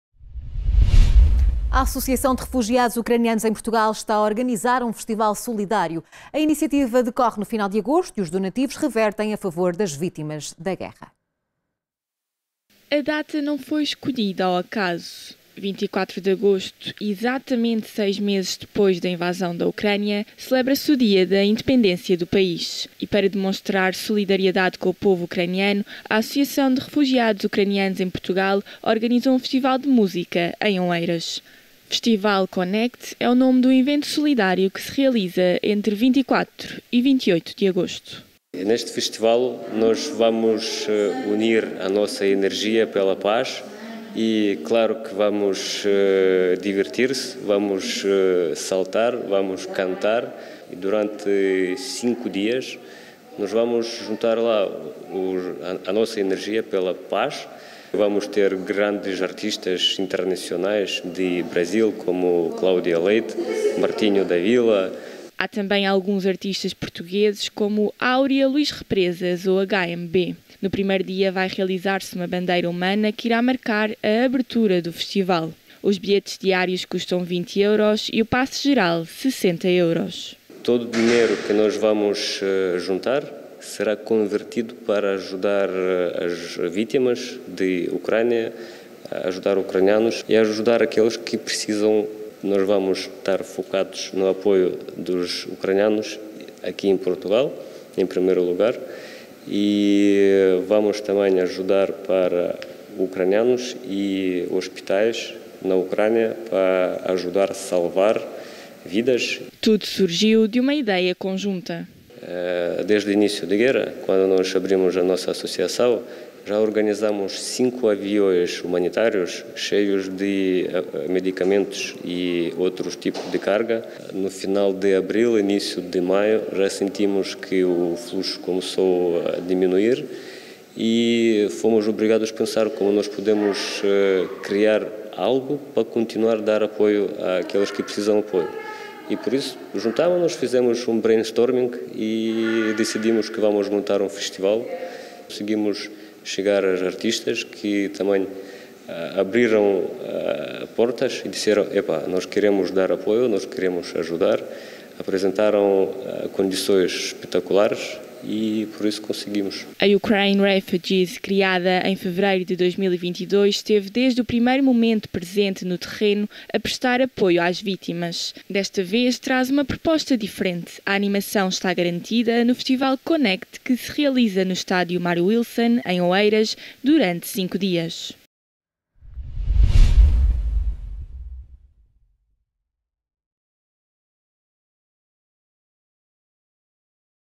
Festival solidário para ajudar a Ucrânia
A Associação de Refugiados Ucranianos em Portugal está a organizar um festival solidário. A iniciativa decorre no final de agosto e os donativos revertem a favor das vítimas da guerra.